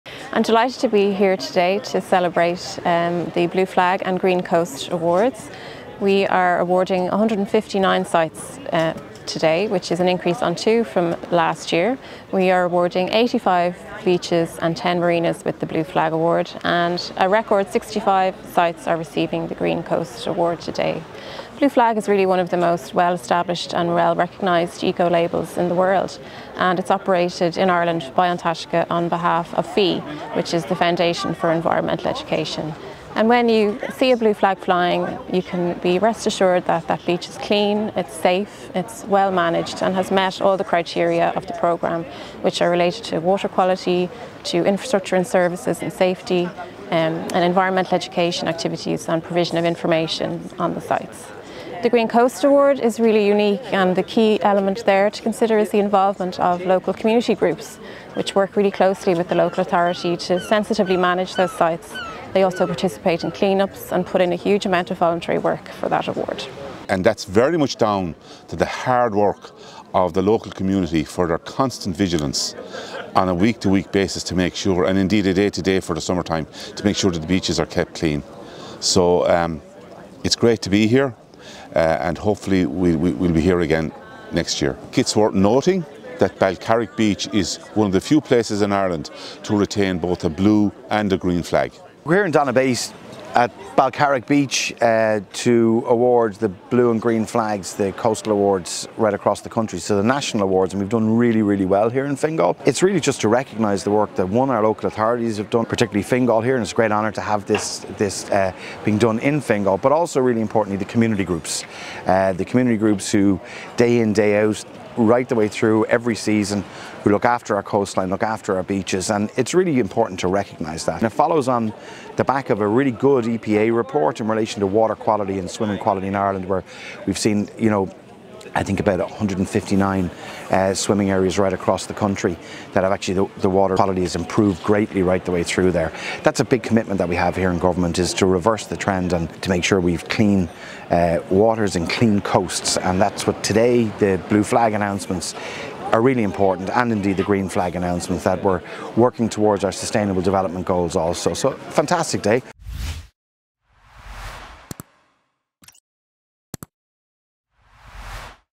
Fingal Beaches retain Blue and Green Flags for 2023
The best-quality beaches along the Fingal coastline have been announced by An Taisce at this year’s National Blue Flag and Green Coast Awards event held at Donabate.

Balcarrick Beach in Donabate, where the 2023 flag ceremony took place, has a 3.4km stretch of sand for beachgoers to enjoy along with great rockpools and a healthy bank of sand dunes to explore. This is the second year that the beach has been awarded both a Blue Flag and a Green Coast Flag, making it one of only eight locations in Ireland to get a double nod from An Taisce.

Across Fingal, there were three Blue Flags and one Green Coast Flags retained, with Velvet Strand in Portmarnock and South Beach in Rush being honoured alongside Balcarrick. They are among 94 blue flags and 65 green coast awards sites around Ireland that have met the most stringent criteria.

The Blue Flag scheme, now in its 36th year, is a prestigious, international award that aims to provide an assurance to tourists that a beach has met a minimum standard. To collect a Blue Flag any Ireland beach must satisfy standards covering the highest quality in water, facilities, safety, environmental education and overall management

Green Coast awards aim to recognise beaches that have excellent water quality and appropriate management to ensure the protection of the natural environment. They have particular emphasis on community involvement and all beaches must be managed by the local authority in cooperation with local clean coast groups.

To find out more about Fingal’s 88km coastline stretching from Howth to north of Balbriggan, go